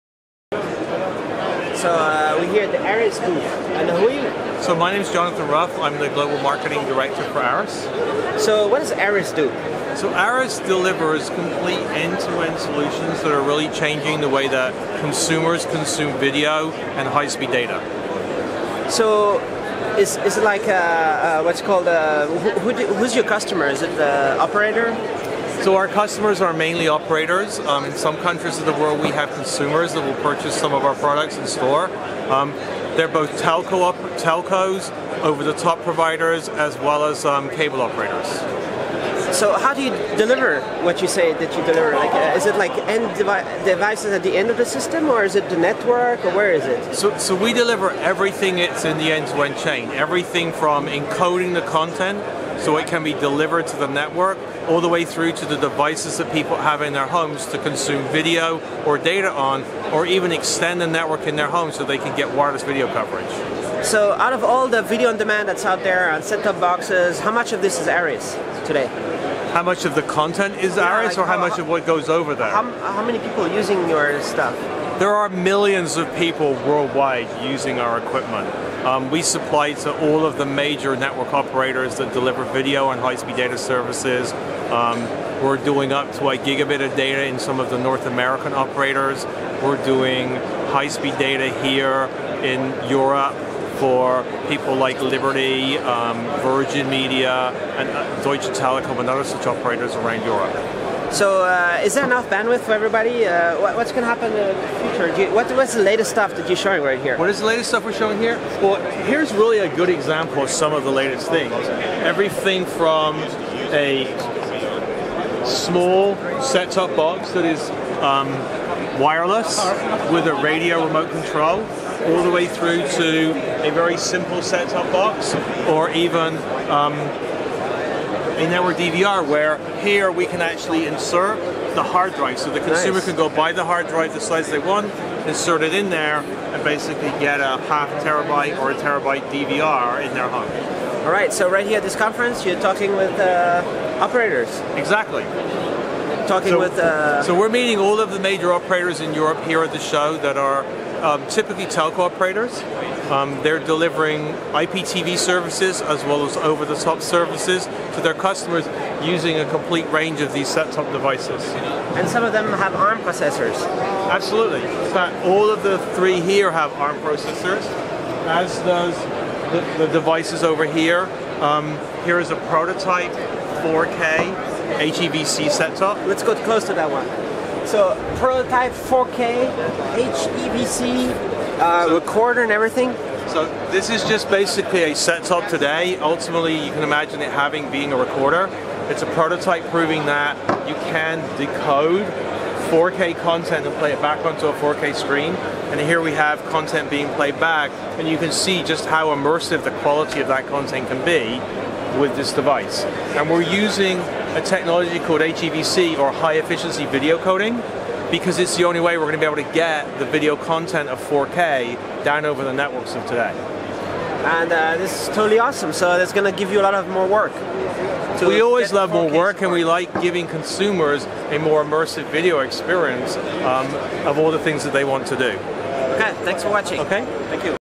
ARRIS Group telecom equipment provider makes Set-top-boxes
Arris bought Motorola's Set-top-box business from Google in 2013 for $2.35 Billion, Arris is a major Set-top-box provider, making several solutions for Set-top-box systems worldwide. Some of the innovations from Arris according to their include
Invented digital TV,
First wireless broadband gateway
First set-top with HD and DVR
First headless, whole-home video gateway
First widescale CMTS deployment
First MPEG-2 and MPEG-4 digital ad insertion
Pioneered security and digital rights management
Recognized with 15 Technical Emmy® Awards